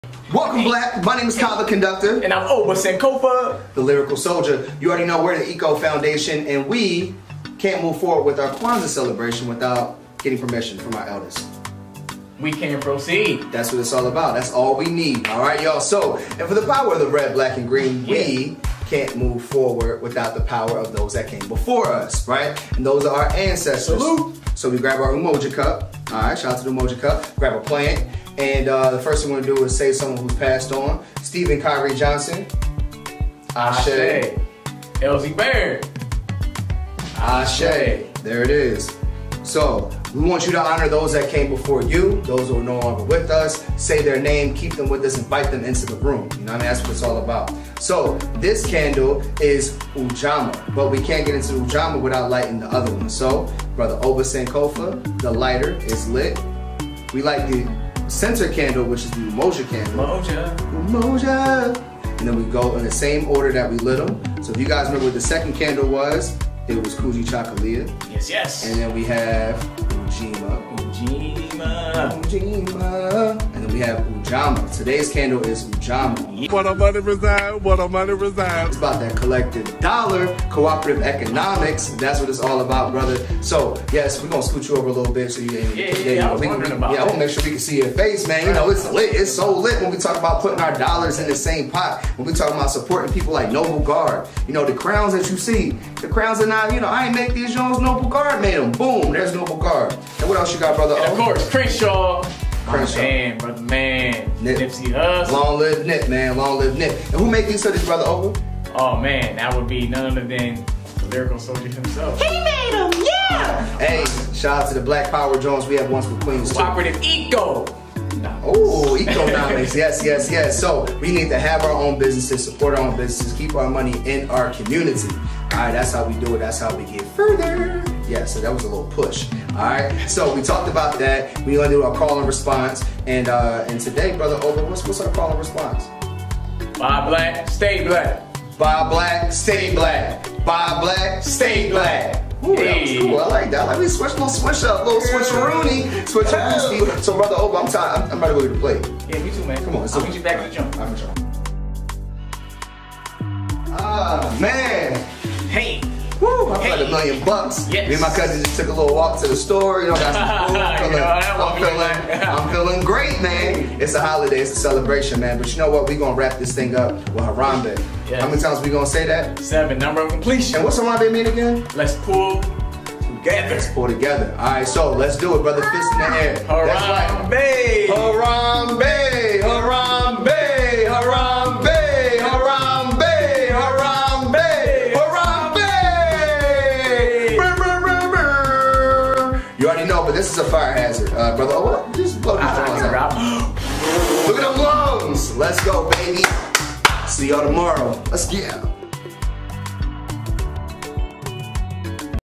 The ECO Foundation Kwanzaa Talkthrough: Ujamaa
The ECO Foundation’s Kwanzaa: Ujamaa

First time celebrating Kwanzaa? TheECOfoundation got you covered...

Let us know how you’ll implement Ujima 365!

Find out more about our organization by visiting TheECOfoundation.com

Consider making a donation at the link below, so we can continue to make cultural content!